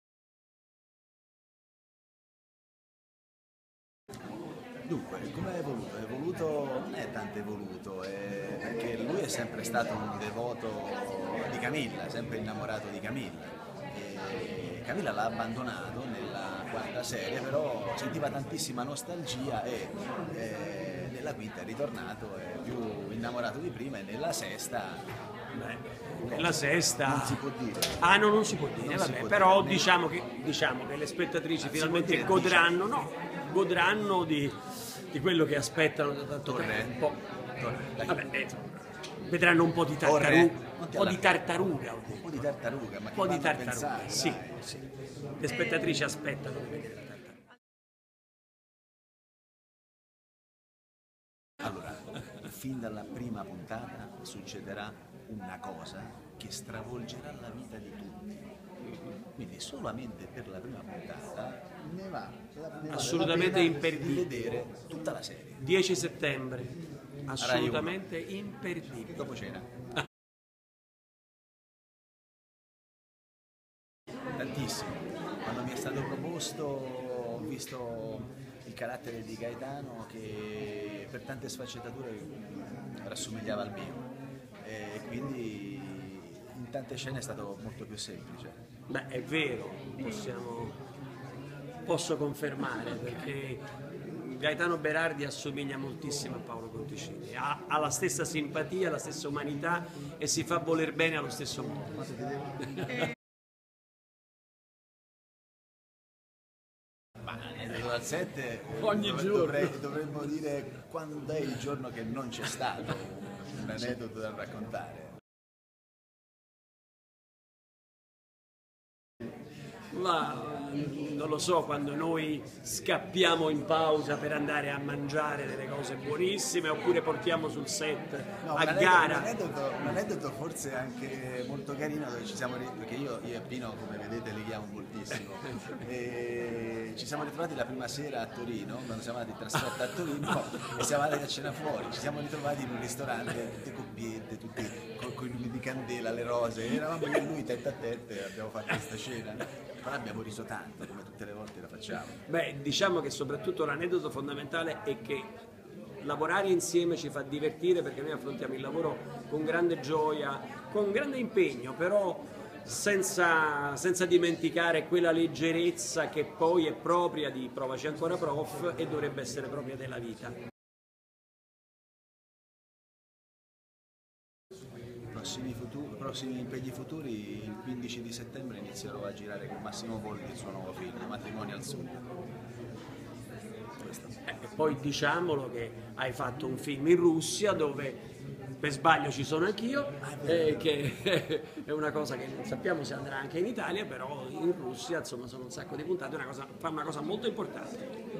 Provaci ancora Prof 6, intervista (doppia) a Paolo Conticini e Pino Ammendola
Intervista doppia a Paolo Conticini e Pino Ammendola, protagonisti di Provaci ancora Prof che ha esordito con la sua sesta stagione il 10 settembre su Rai 1. Paolo Conticini, con l'aiuto amichevole di Pino Ammendola, risponde alle nostre domande sul futuro di Gaetano, gli aneddoti dal set e i progetti futuri.